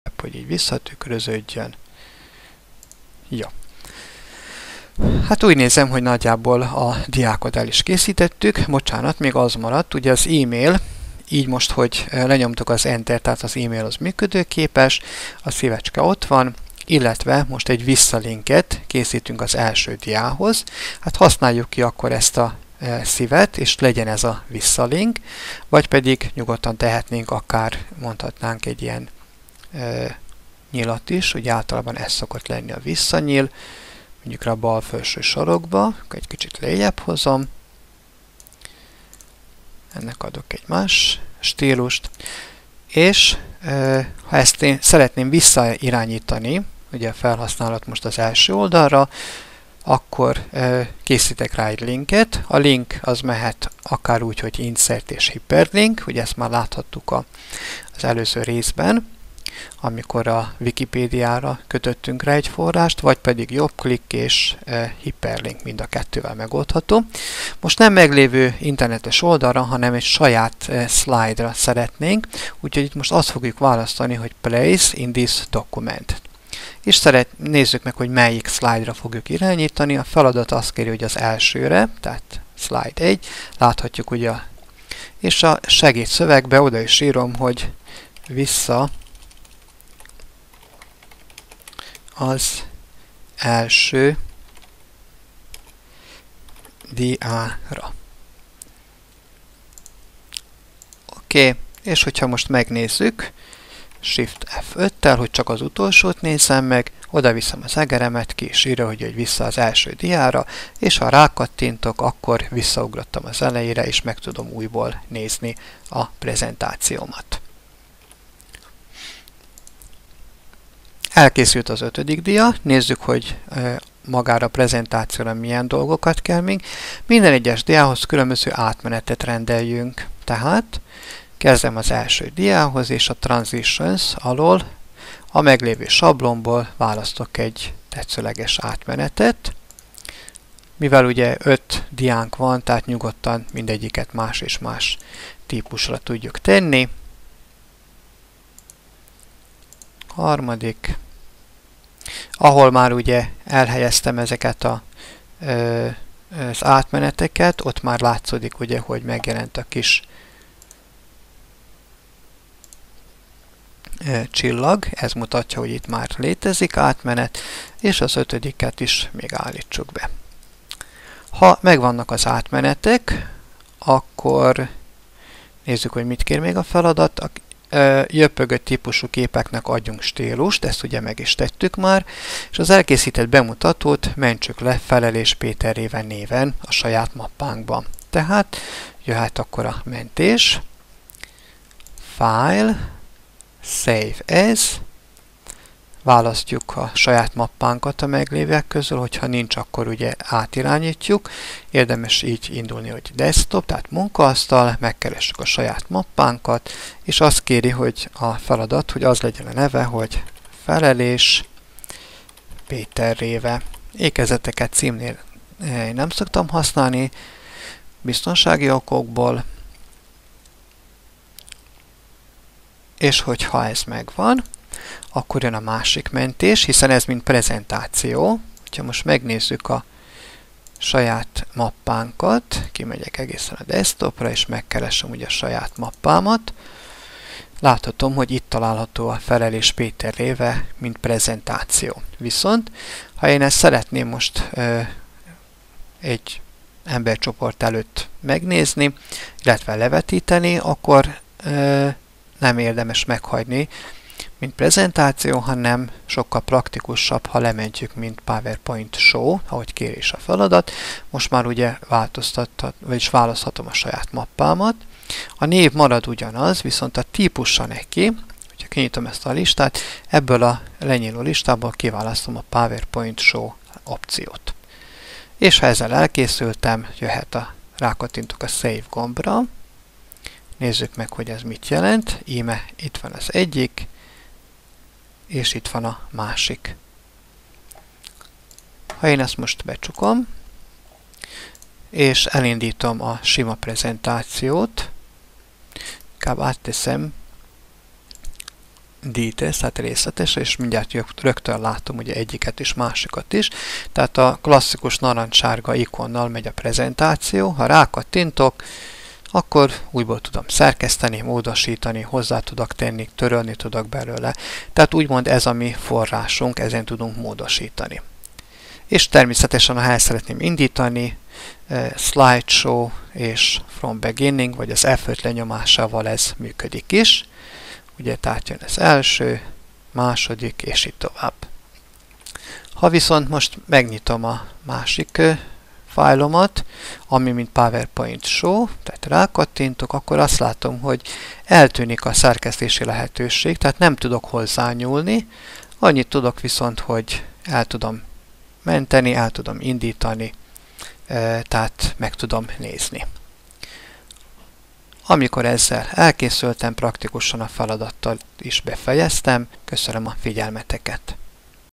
BEMUTATÓ KÉSZÍTÉSE (ÁTMENETEK, DOKUMENTUM MENTÉSE)
Ebben a videó sorozatban megismerkedünk a bemutató készítés alapjaival egy konkrét feladat kidolgozásán keresztül.
A felhasznált elemek: cím, elrendeződés, háttér, szöveg írása, másolása az internetről, képek beillesztése, kép másolása az internetről, effektusok, animált képek, átlátszó hátterű képek, élőlink használata, átmenetek, mentés mint dokumentum, mentést mint slide show